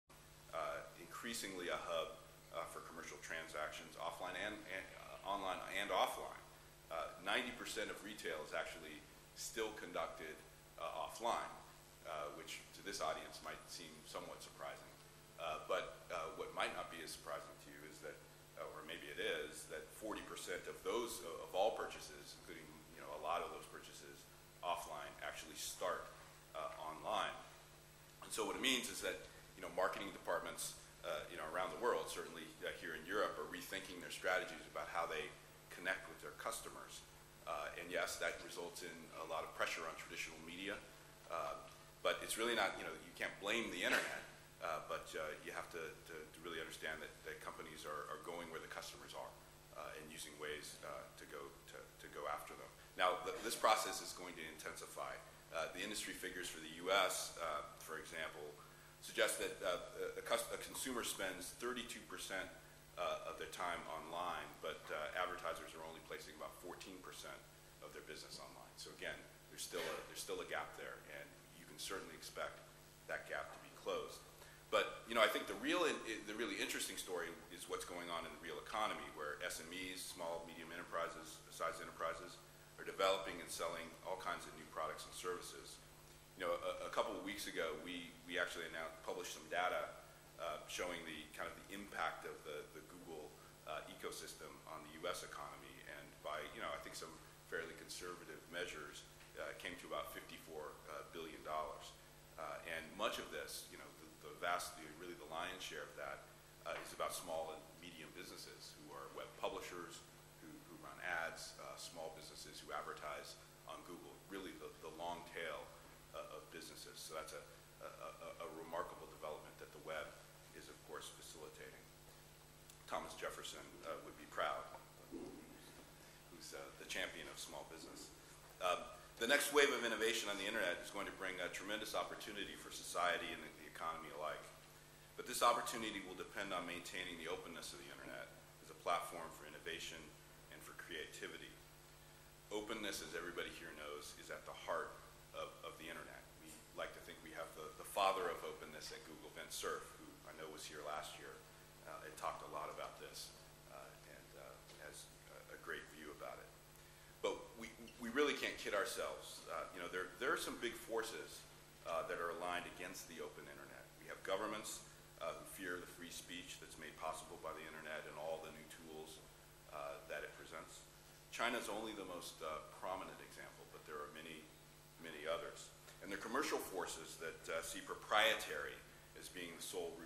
Openforum europe - Summit 2010 Session 1 pt3
David Drummond, Senior VP Corporate Development and Chief Legal Officer, Google